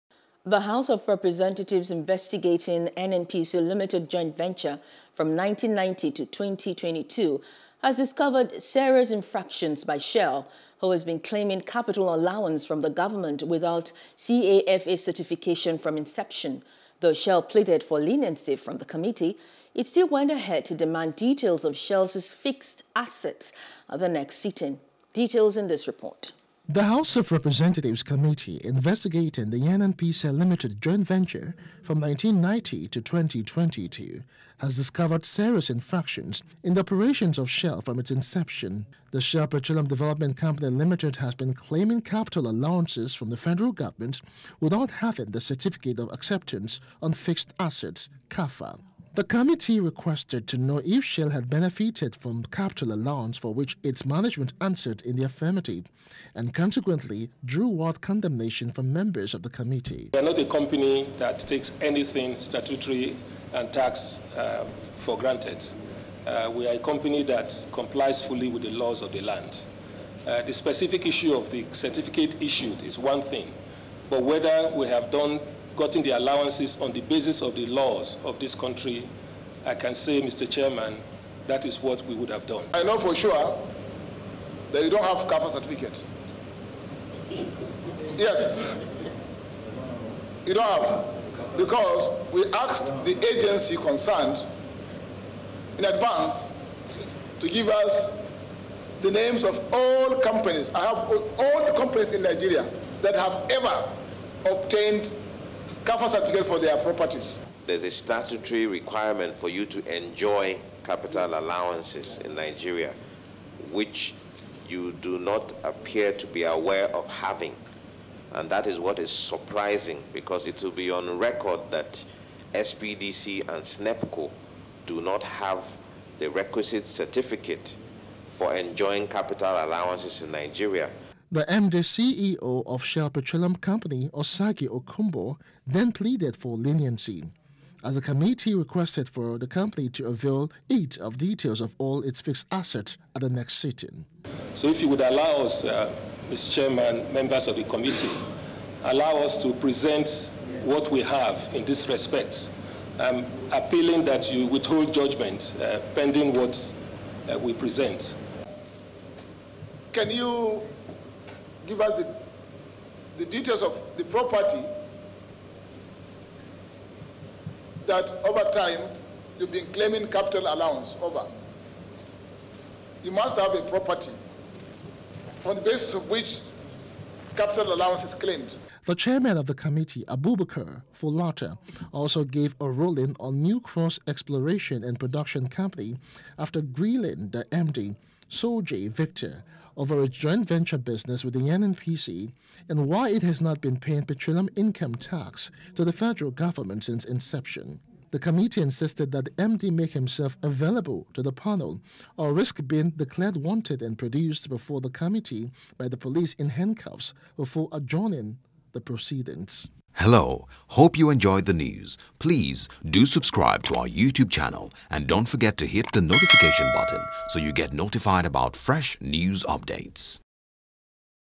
NNPC Joint Venture: Shell Defrauding FG By Claiming Capital Allowance Without CAFA Certificate |NEWS
The house of representatives investigating NNPC Limited Joint Venture from 1990 to 2022 has discovered serious infractions by Shell who has been claiming capital allowance from the government without CAFA certification from inception.